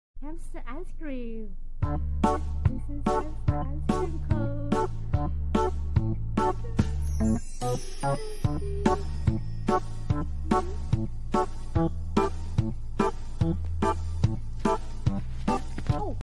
Hamster Flavored Ice Cream
What's your favorite ice cream flavor??? ^^

Don't worry, guys. She fell down on the coach. She wasn't hurt.
Later, I gave her the whole cone, and she liked it  ^^